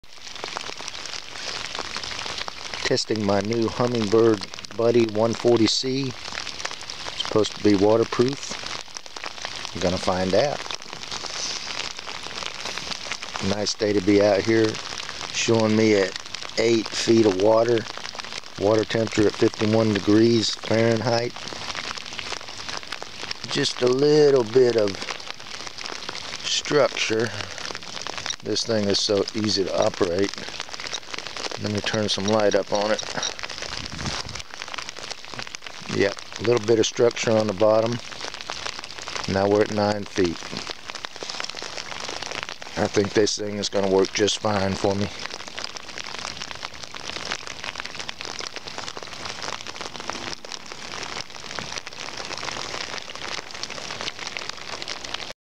MVI 8658 3-16-2014 testing my new Hummingbird Buddy 140c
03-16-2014 testing my new Hummingbird Buddy 140c. supposed to be waterproof :) 43 degrees, rain, east wind at 10-20 mph. she did well